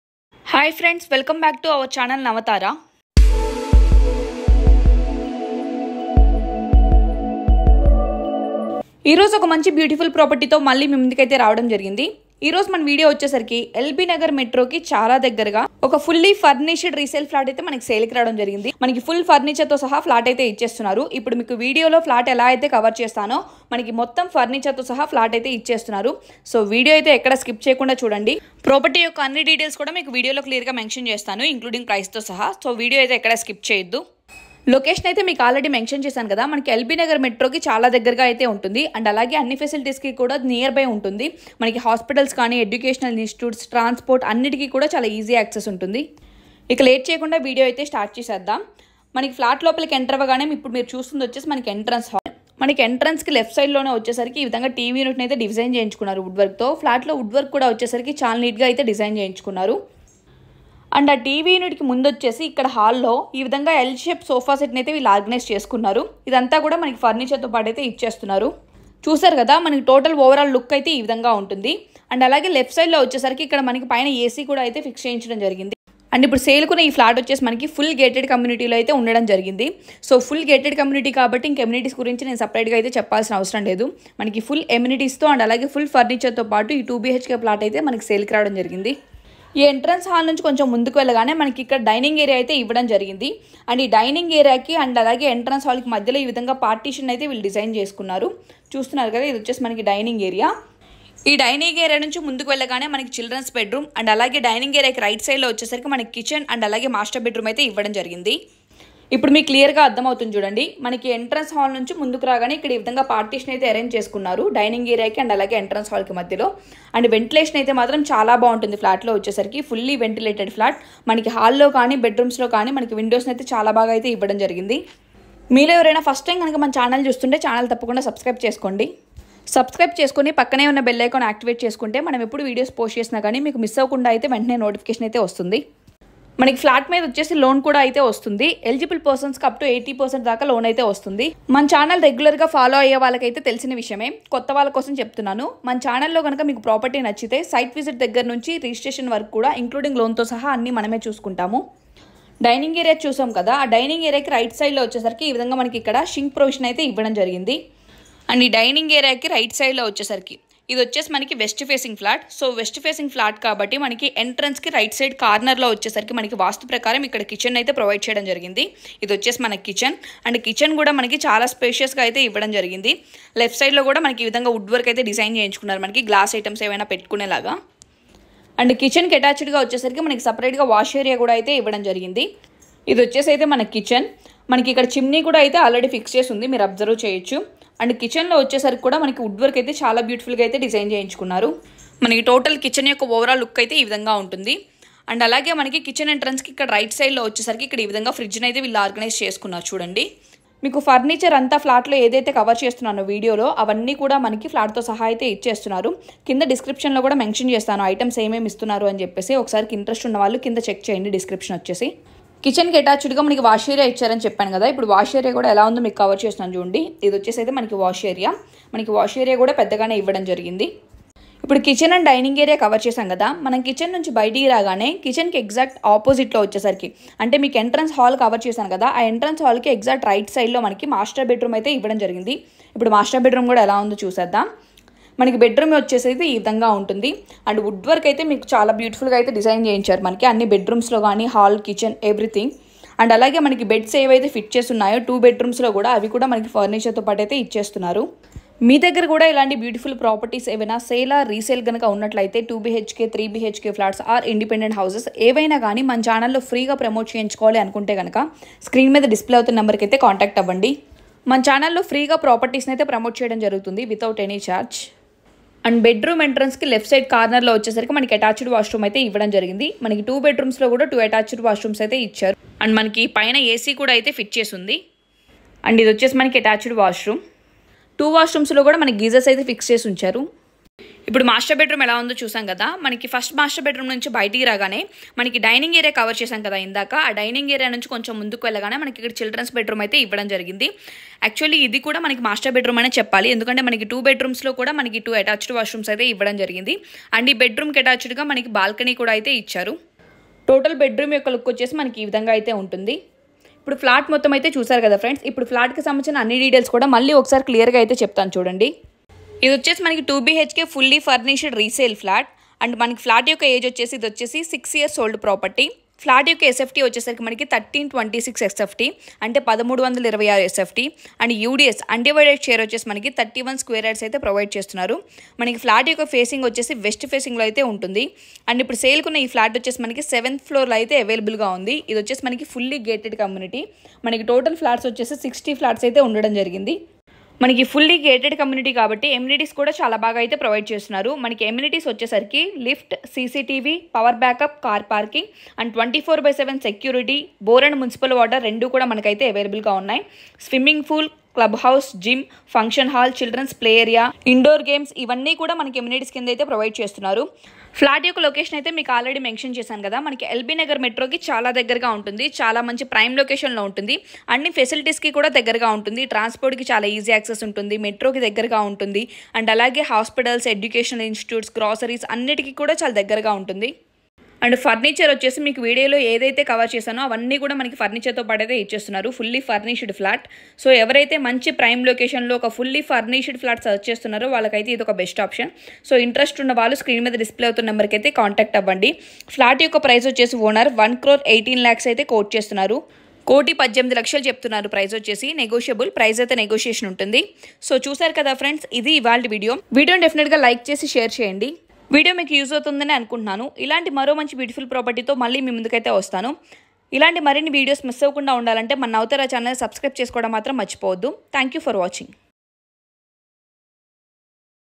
(P-171)Fully furnished Resale Flat for sale in Hyderabad|very near to Metro and Main Road|prime area
Navataara properties
independent Houses
Resale Flats
Brand New flats
Open Lands
Home Loans

Type : 2BHK Resale Flat For Sale in Fully gated community.
SFT :1326 SFT ,West Facing.
UDS : 31 UDS SQ YDS Land.
Floor :Available in 7th Floor
Per floor 06 flats only.
Total Flats in apartment 60 + Flats
Age: Just 06 years only
Loan : Available Up to 80%.
possession : Ready to occupy.

Amenities: car parking,two wheeler parking, Lift, CCTV,24/7 Water facility available, Manjira water facility available.
Walking track
Swimming pool
Clubhouse
Party hall

List of furniture:

Sofa set
Dining table along with the chairs
Geyser in bathroom
Beds in bedrooms
AC
Fans
Note : As per video all items they will provide.
Location: walkable Distance to Main Road and LB Nagar Metro .

(P-165) New furnished 3BHK Flat for sale in JNTU Hyderabad |1910 SFT| Very near to Metro station .